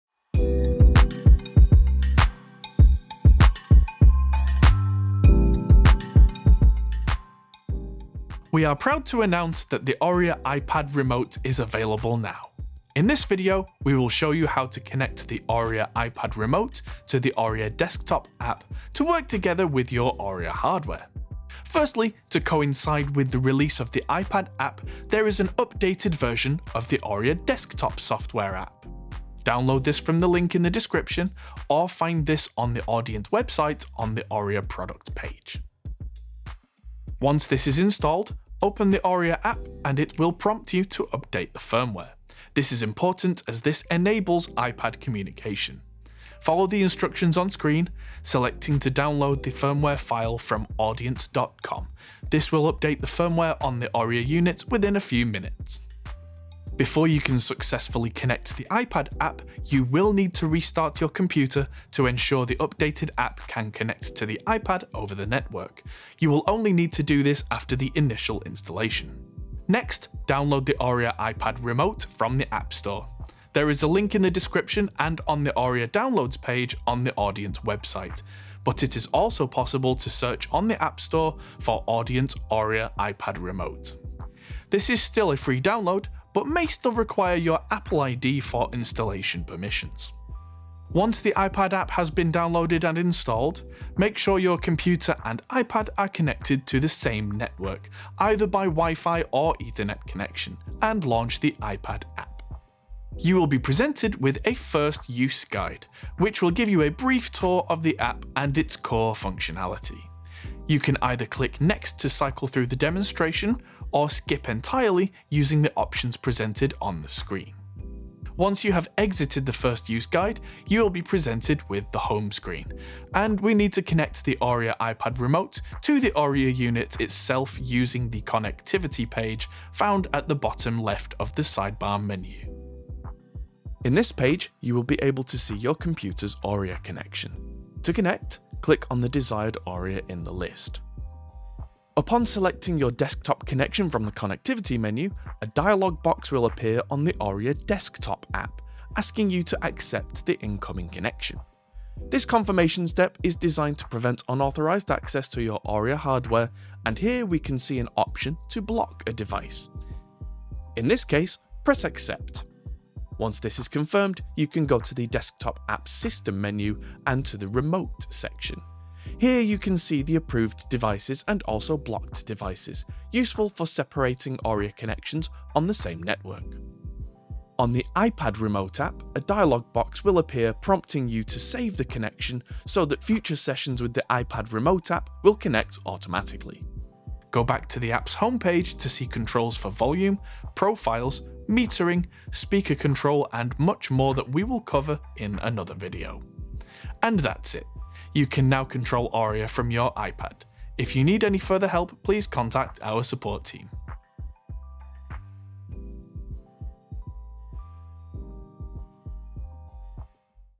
ORIA iPad Remote - Quick Start
In this quick start video, we’ll guide you through the essentials of connecting and setting up the ORIA iPad Remote App. Expect a straightforward walkthrough of initial setup steps, including pairing the app with ORIA and configuring basic controls to get you up and running smoothly.

What is ORIA?
ORIA is the world’s first audio interface and monitor controller designed specifically with immersive audio in mind, allowing you to calibrate, control and monitor speaker formats from stereo all the way up to a 9.1.6 configuration.

In this video, we walk you through how to get set up with the connection between ORIA and the iPad Remote App.